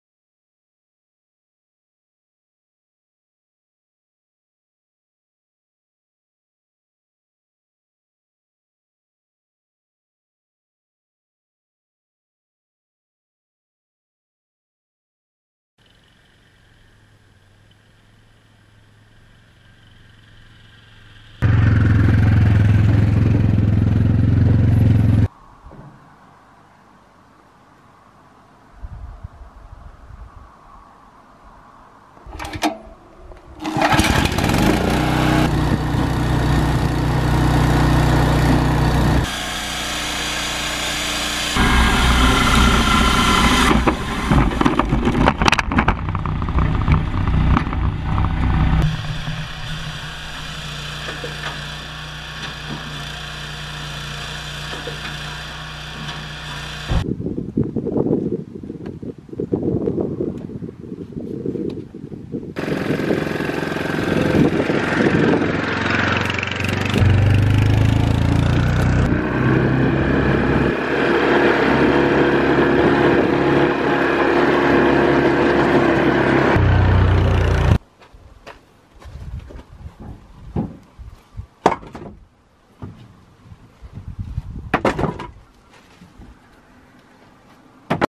Firewood processor with Yamaha and ATV trailer
We were making some firewood using Japa 305, Yamaha ATV and our own ATV trailer.